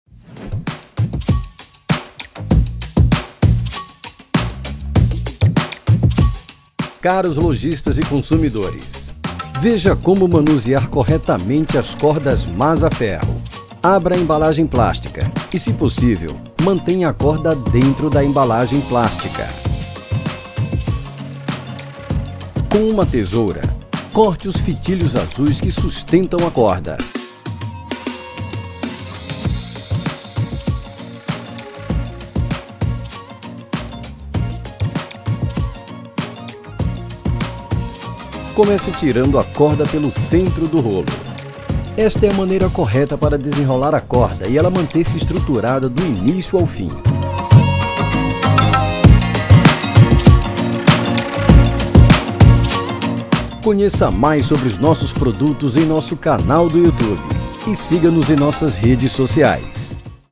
Como manusear CORRETAMENTE as CORDAS MAZZAFERRO!
Você sabia que existe uma forma simples e correta para manusear as cordas Mazzaferro no seu PDV?